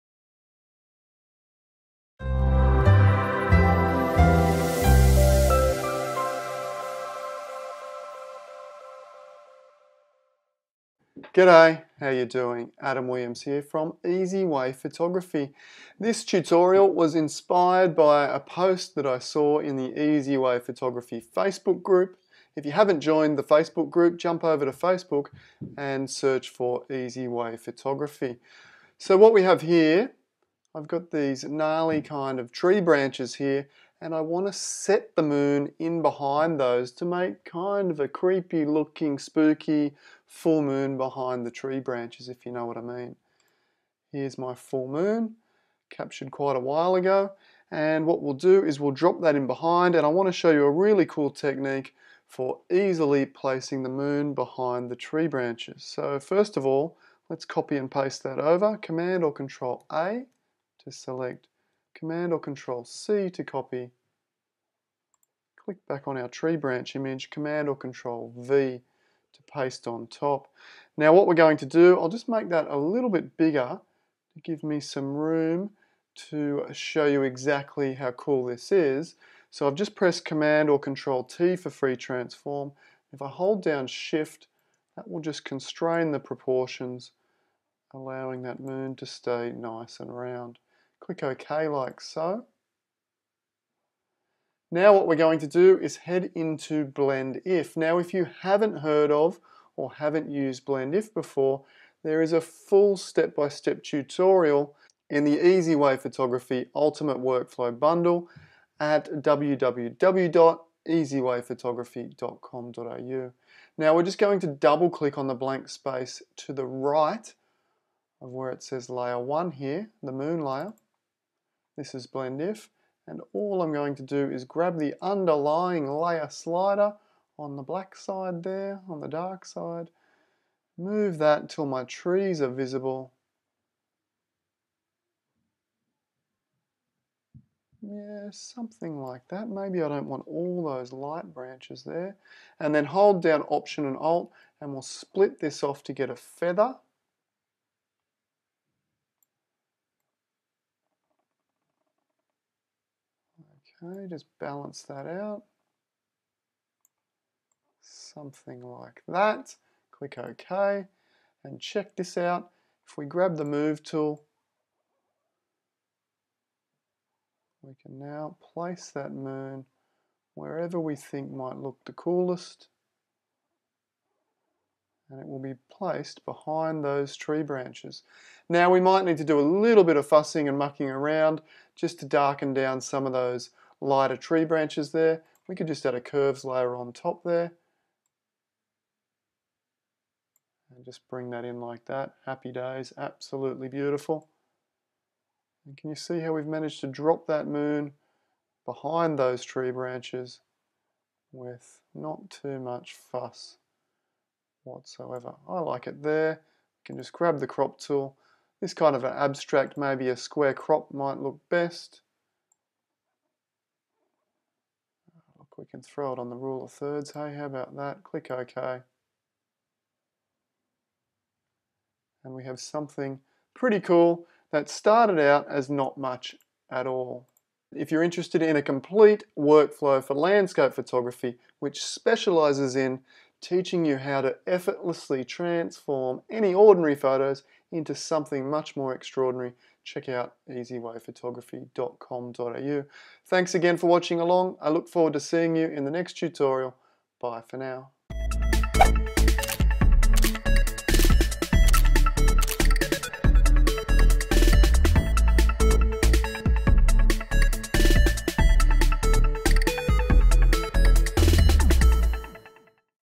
Add a Full Moon the Easy Way
🔥 Click the link below for a FREE Photoshop Course for beginners -

How to add a Full Moon the Easy Way

How to use the power of Blend If to drop a full moon seamlessly behind tree branches!

Recommended video: "Transform any Ordinary landscape photo into something Extraordinary!"